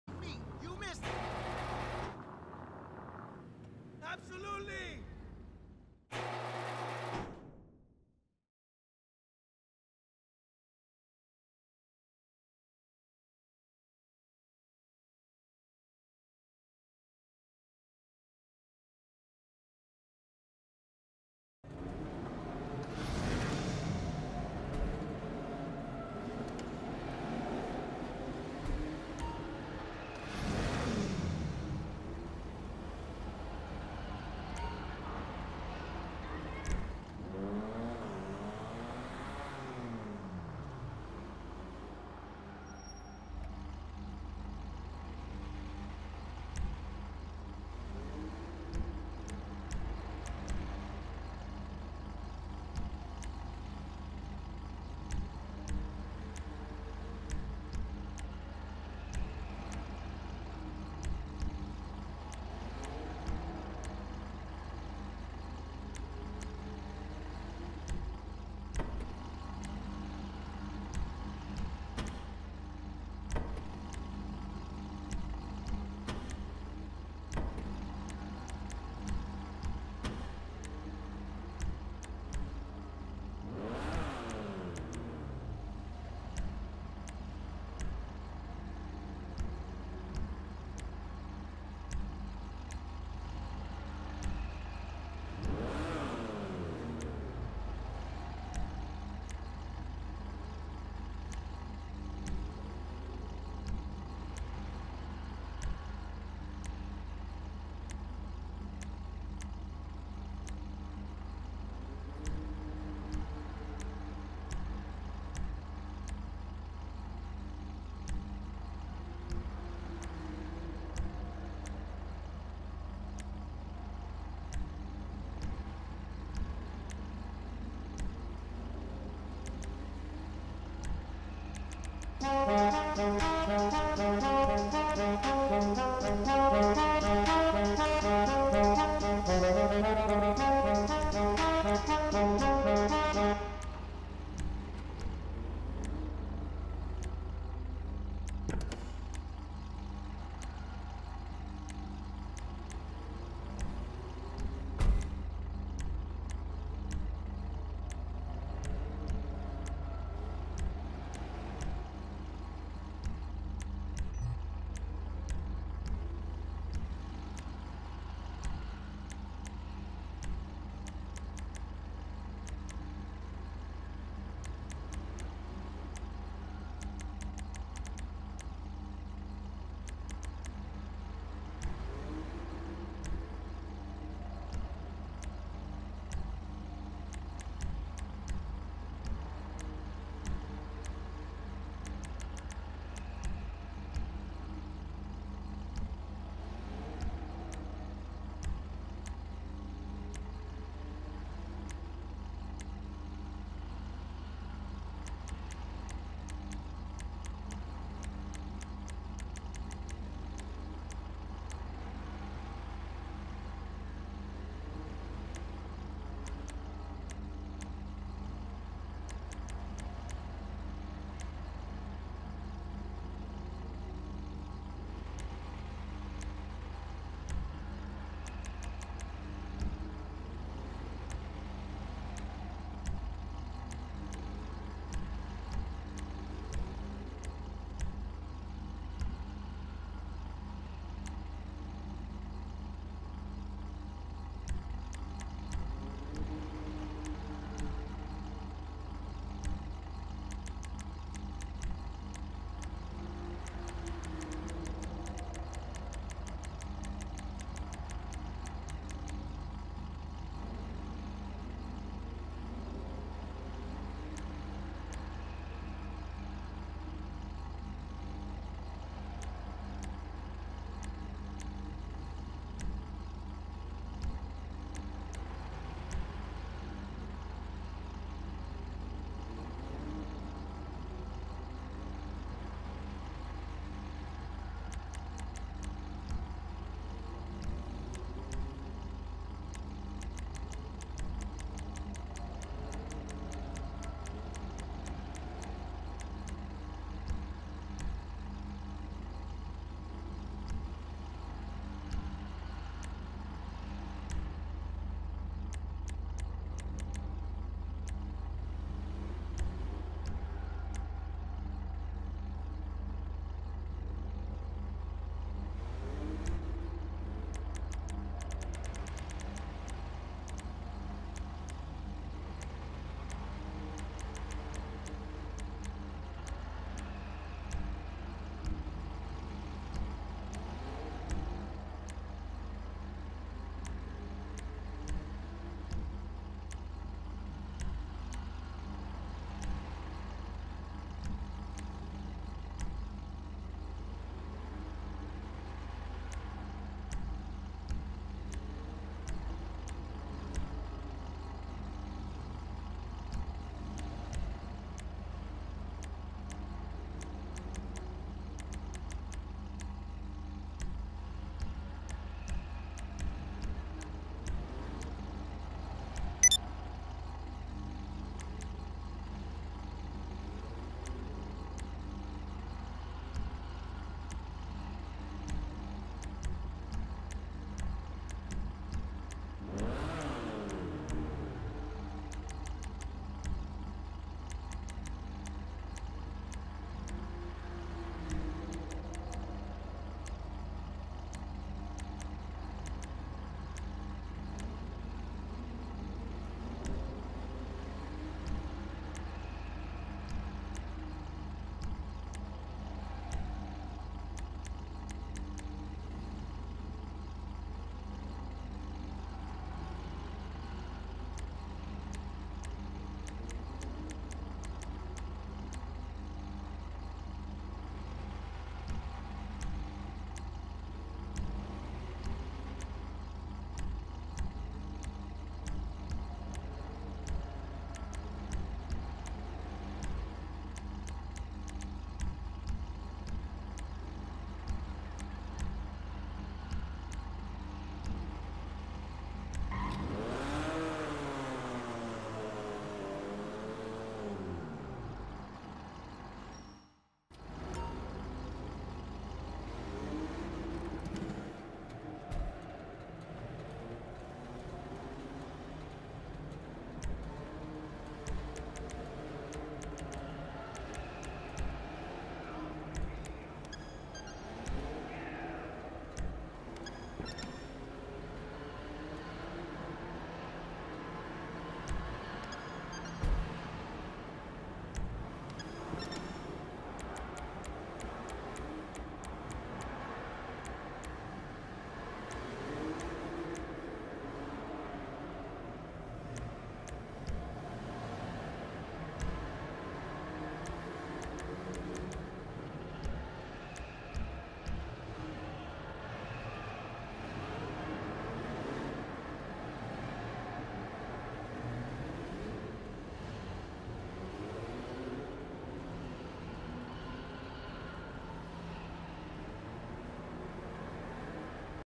Annis ZR350 Upgrade
Customizing the Annis ZR350. Upgrade and walk through of a full customization of the digital car.